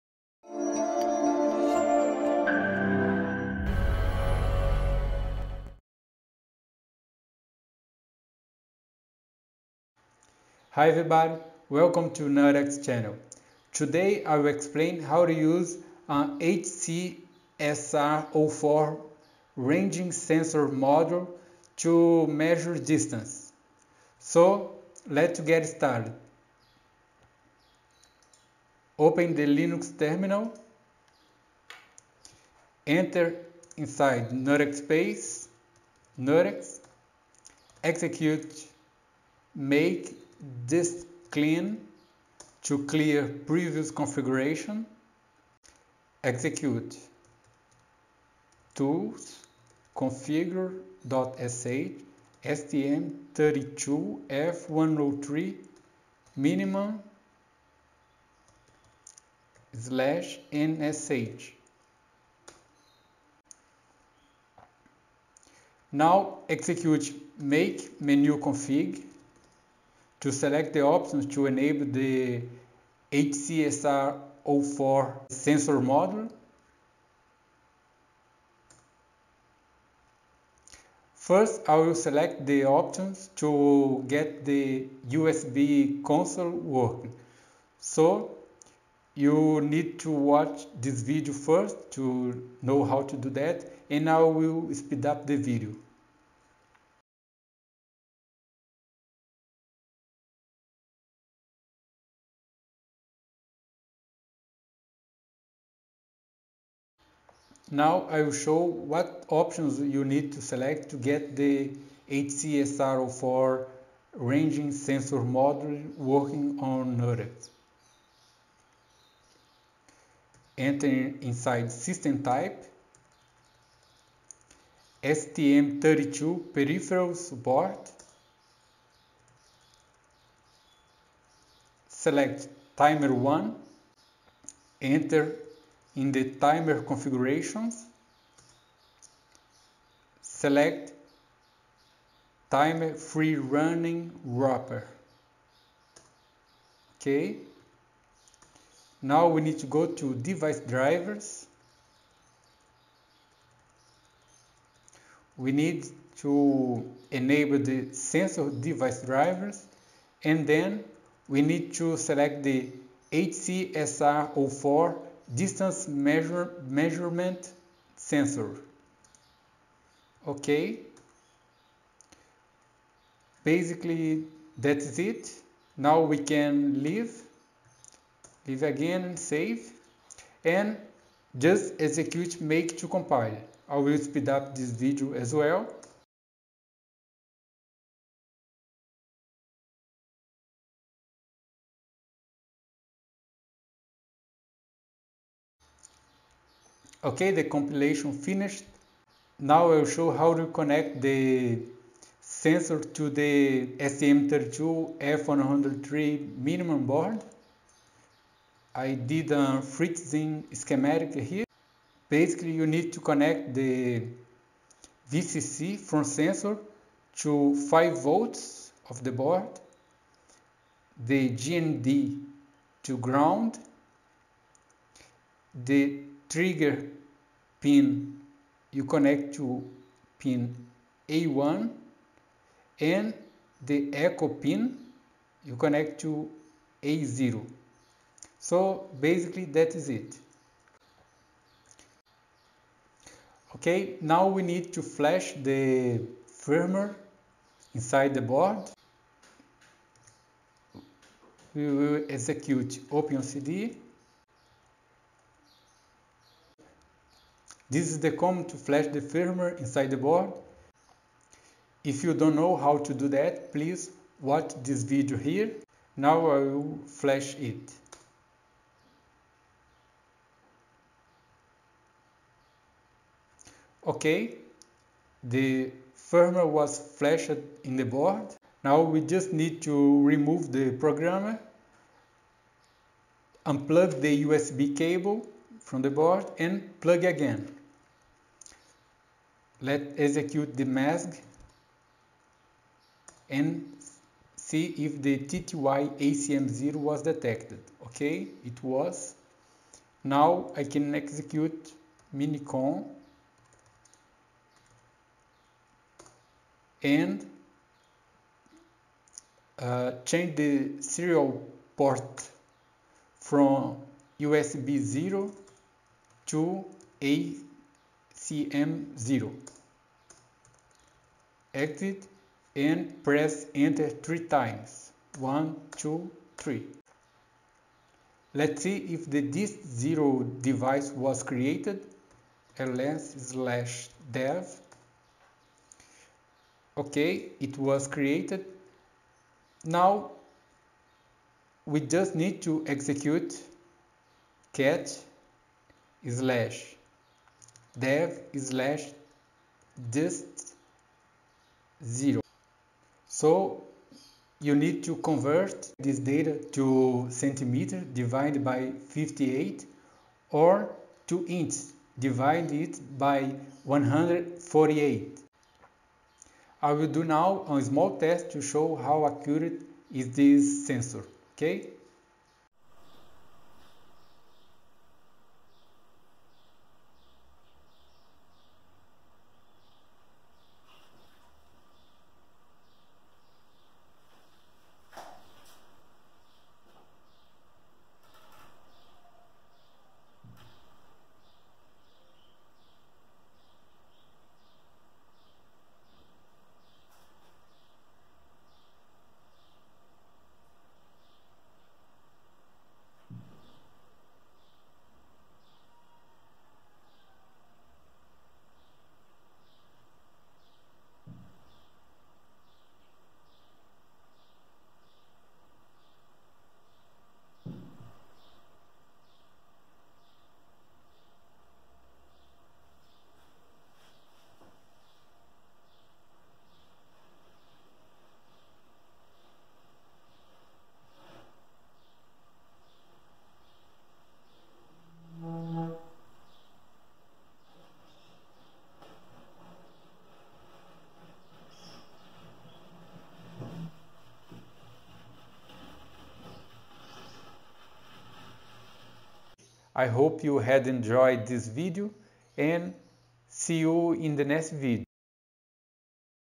#016: Using the HC-SR04 to measure distance
In this video I explain how to use the HC-SR04 ranging sensor module to measure distance. I created the HC-SR04 and submitted to NuttX mainline few days ago. If you want to know how the driver work, please read it